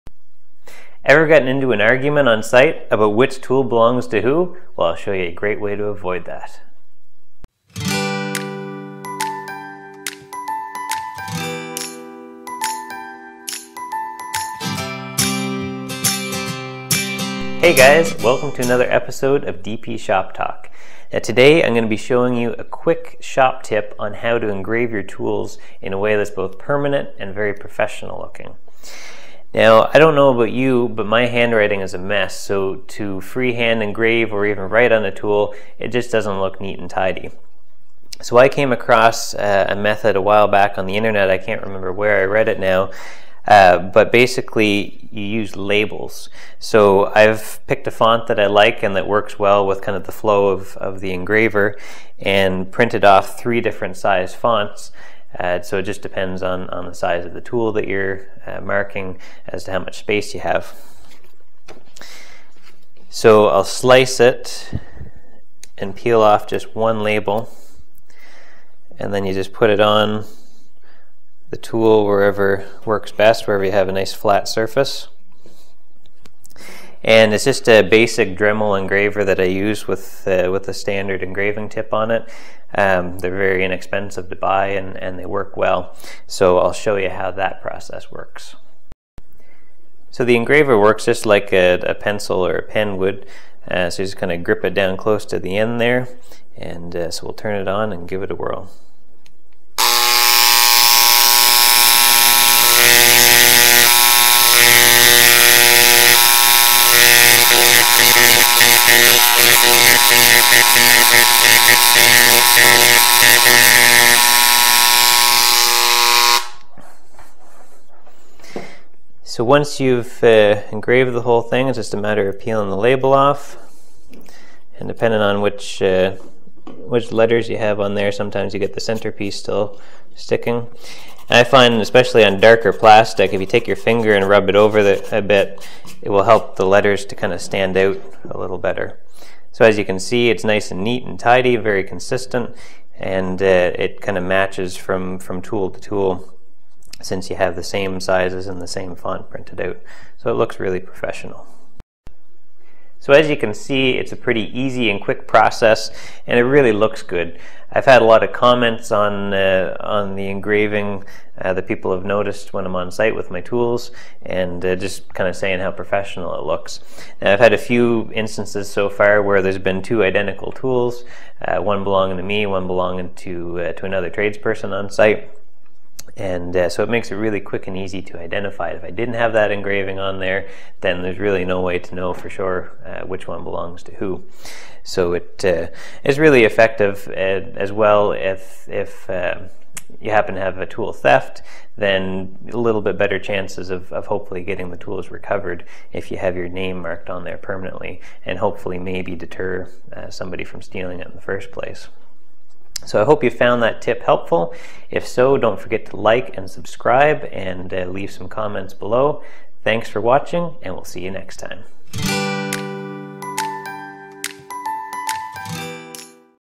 Engraving Your Tools with Professional Results
Learn a quick and easy way to mark your tools or other belongings that looks professional and is consistent from from item to item.

This how-to video shows the quick and easy process that gives you professional looking results for engraving your tools or other belongings.  Engraving helps prevent loss and theft of your tools or belongings and makes them easy to identify as your own.  With only a Dremel engraving tool, a printer and peel & stick labels you can create your name or other personal marking in the font of your choosing and get professional and consistent results.  Engraving is a much more permanent way to mark items that can't be easily wiped off like marker can.  Watch the complete process in this quick and informative video.

To learn more about the Multi Purpose Table (MPT) or to purchase plans please

Follow DP Shop Talk on Facebook and Twitter to stay up-to-date with the latest news!